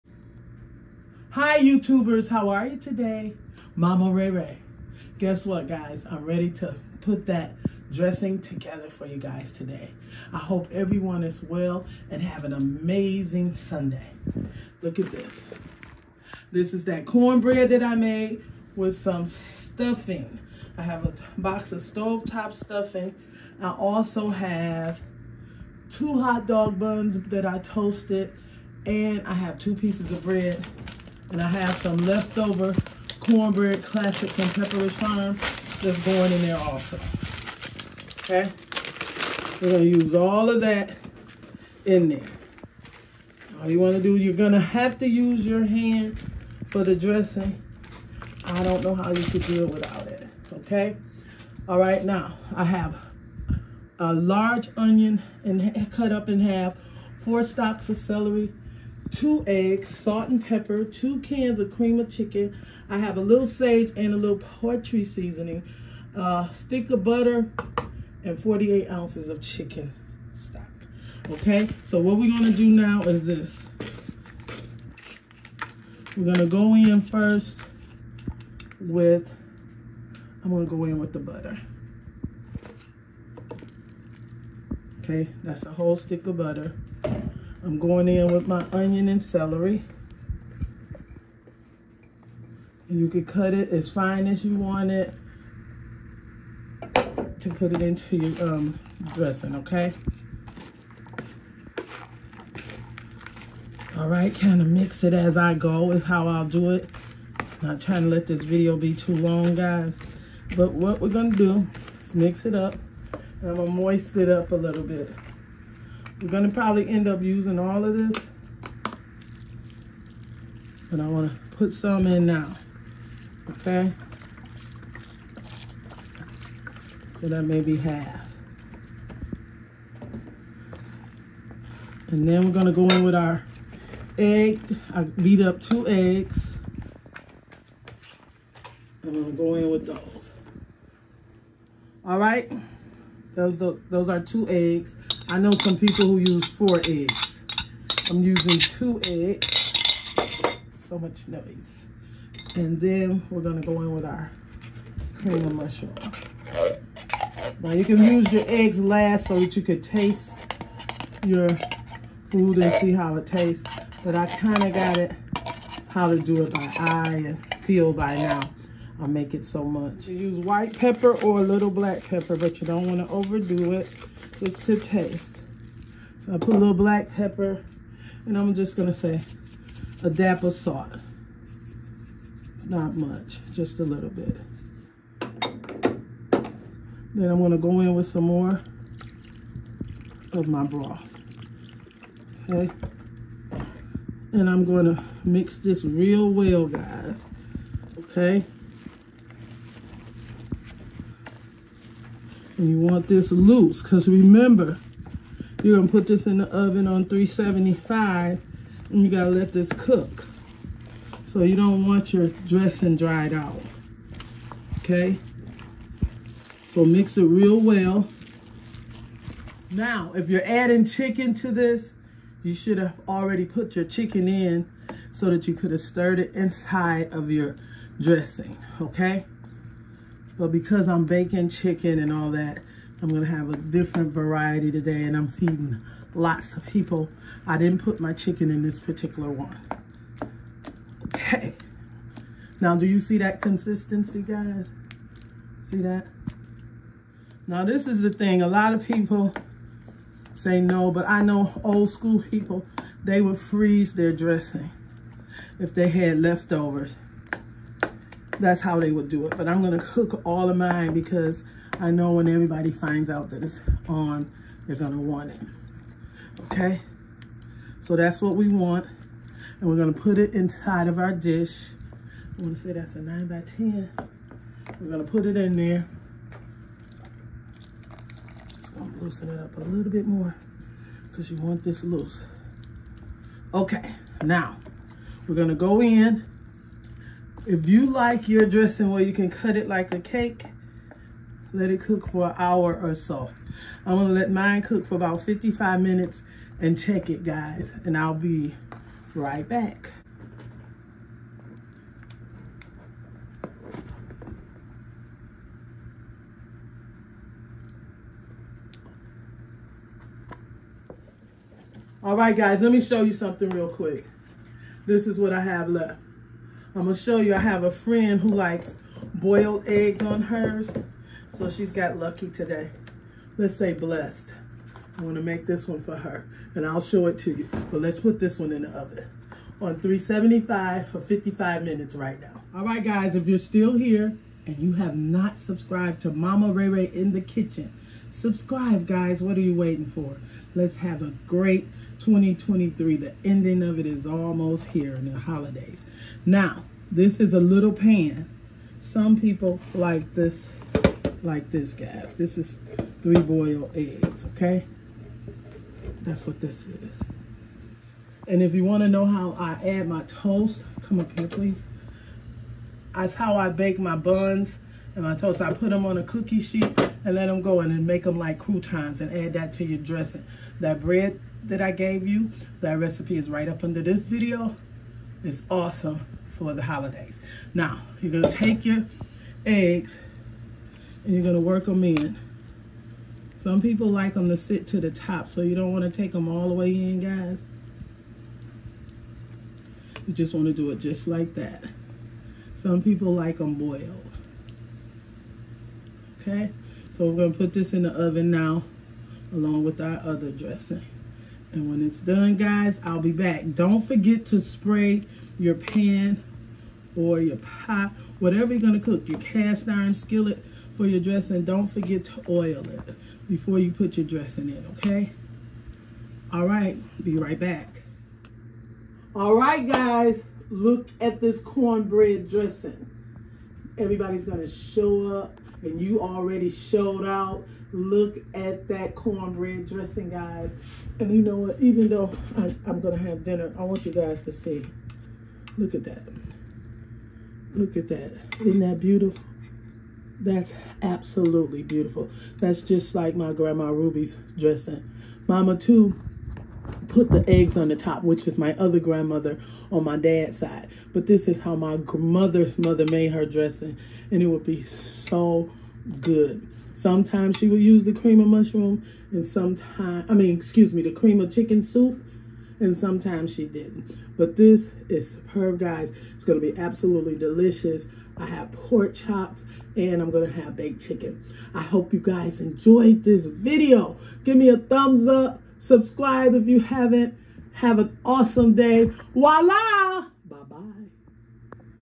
The Best Southern Cornbread Dressing
The best southern cornbread recipe for your cornbread dressing from scratch easy.Hope you give this recipe a try for the holiday.

Ingredients

1 pan of cornbread from scratch
1 box of stove top cornbread
48oz chicken broth
2 can of cream of chicken soup
Salt & Pepper (to taste)
Four stalks of celery (cut fine)
1 whole onion
Two Eggs boiled